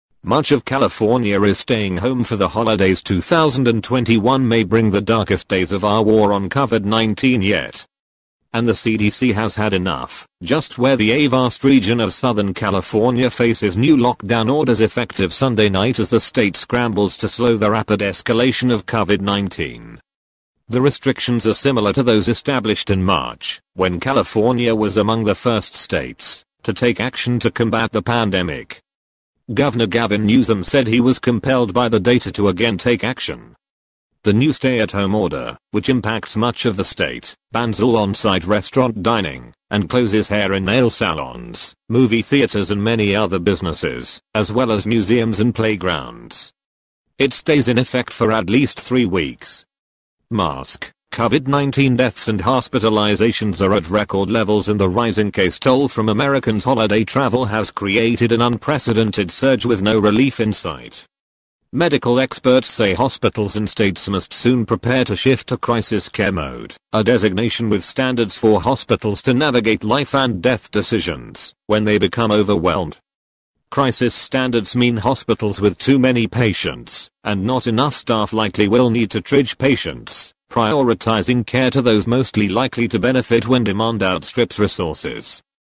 California gov says don't wear a mask? if people wear mask then you have to pay fine🙄🌠globe news
Here's the biggest news you missed this weekend